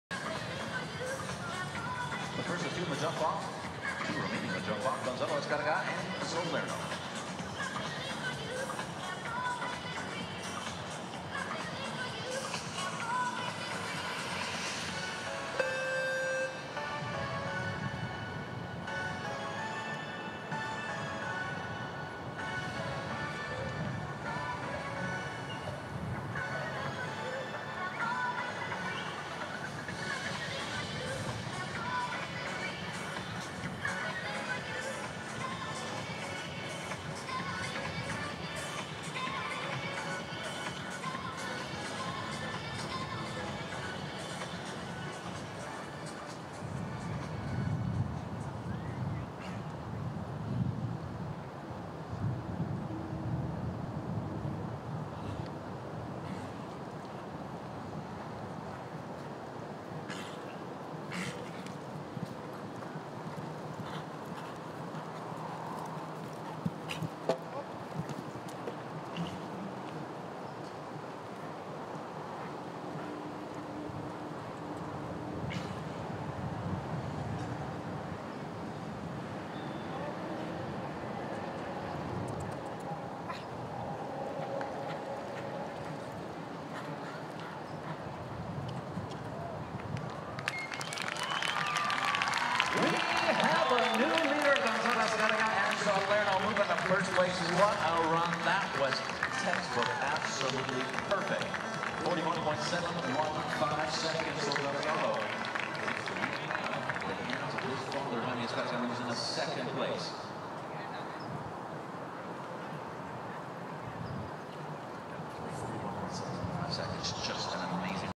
Gonzalo Azcarraga and Zalerno, winners of the $25,000 Markel Insurance Grand Prix
2018 Showpark August Festival, Del Mar Horse Park, courtesy of MyHorse.Video